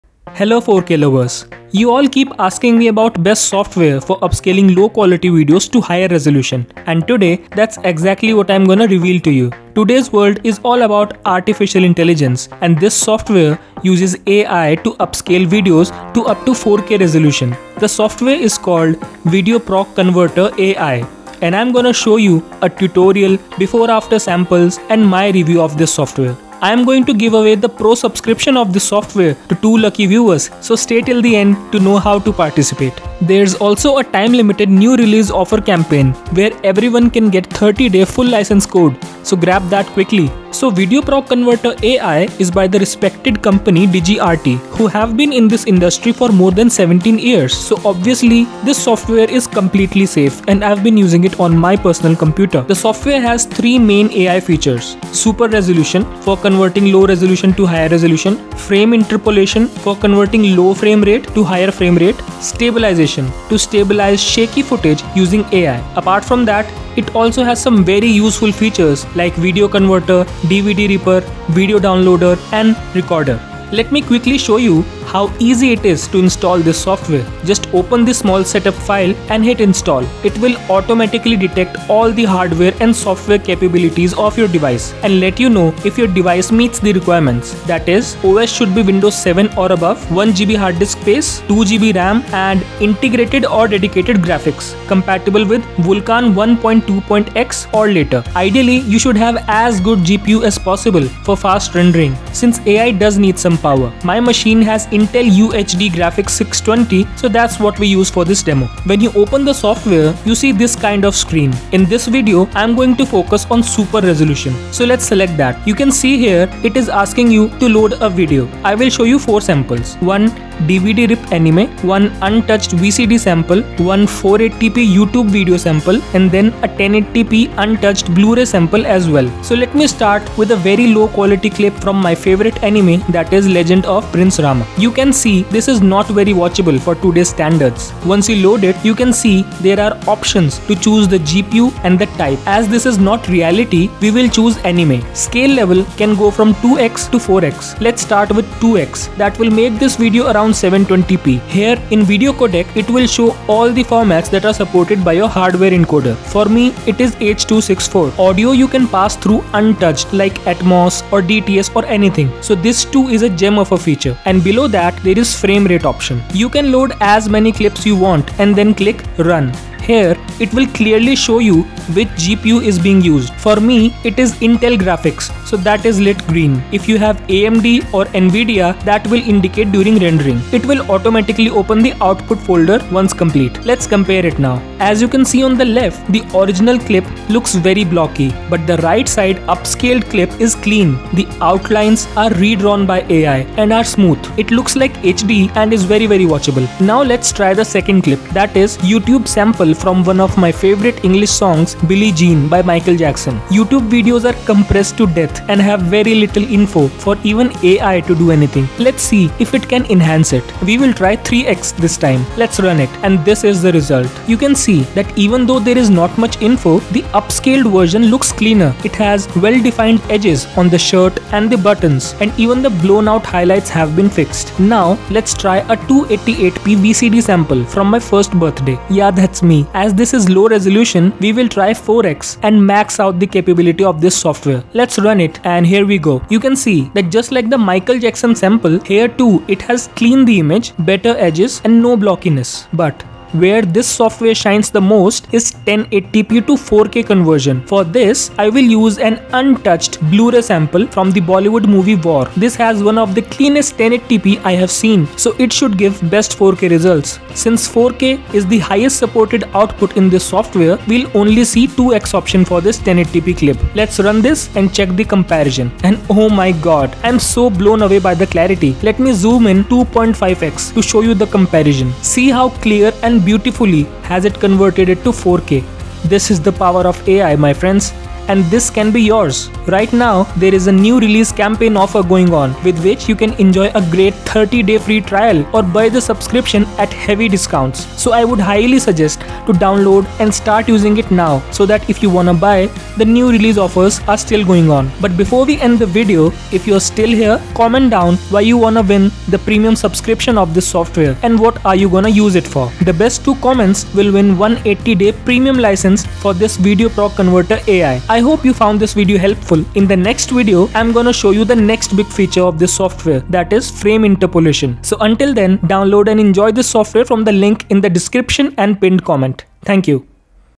Using AI to upscale low quality/old video into 4K - VideoProc Converter AI
Today, I am going to show the tutorial – how to upscale low-quality / low-resolution videos to high resolution. With VideoProc Converter AI, you can enhance old/SD/ low-quality / low-resolution videos & photos; create smoother video with AI-generated realistic higher FPS; convert large/4K/HD/DVD video, edit, compress, download, and record with GPU acceleration.